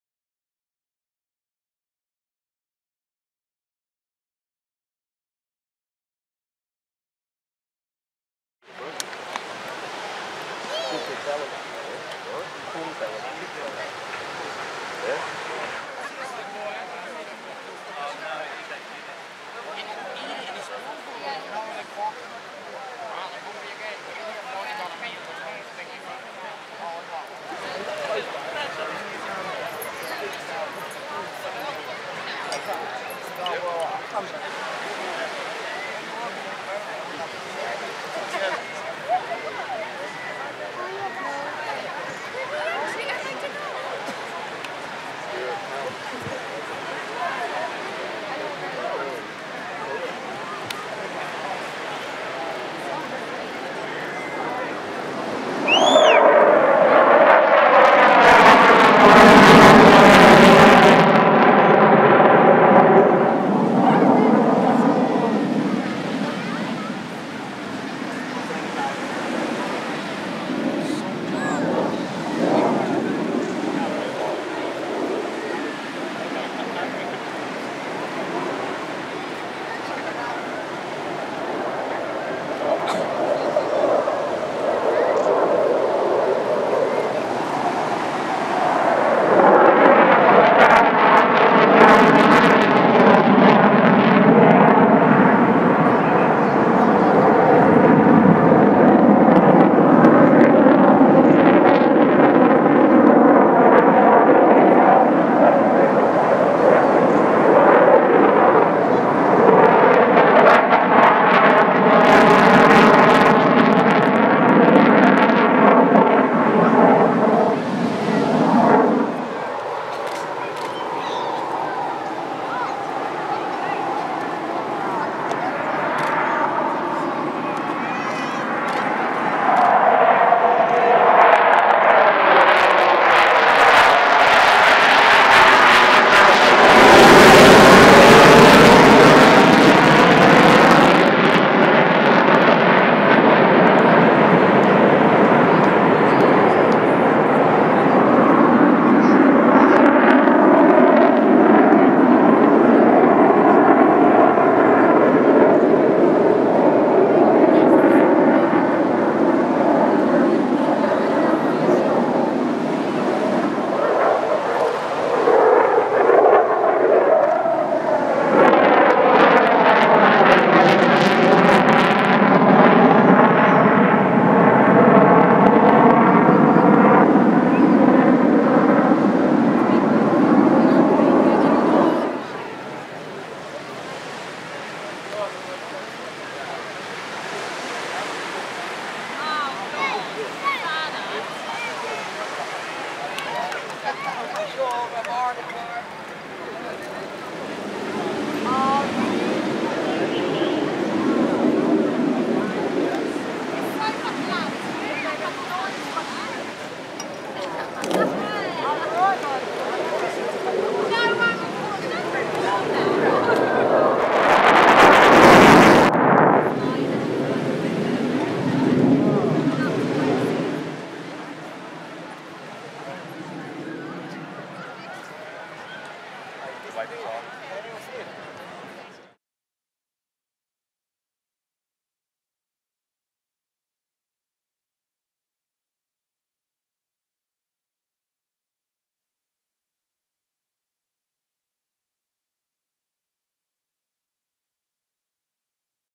RAAF F/A18F Super Hornet Display Mindil Beach Darwin August 2014
Held every two years in Australia's Top End,  the multi-national Exercise 'Pitch Black' plays host to war games sharpening the expertise of all Air Forces involved.
As a thank you to the people's of Darwin,  one of the RAAF's frontline strike aircraft from No.1 Squadron flying their Super Hornet put on a thrilling display as the sun set on another glorious  day  at  Darwin's iconic Mindil Beach, with thousands flocking for the night markets and party like atmosphere , with mother nature topping the show with it's stunning sunset.